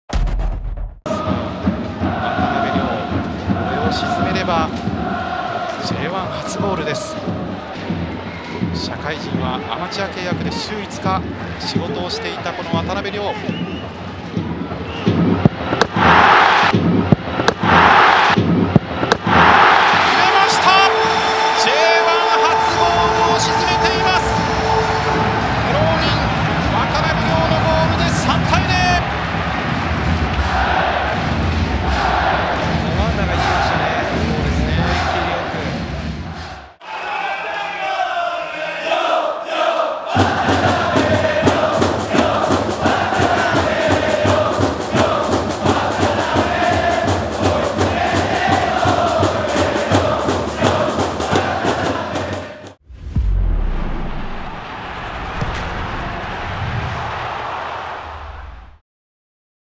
J1初ゴール⚽ 渡邉りょう 選手🌸
【9/2 川崎戦】
J1初ゴールをあげた 渡邉りょう 選手⚽
試合後サポーターから祝福を受ける様子をお届け👏

りょう、初ゴールおめでとう🌸🌸🌸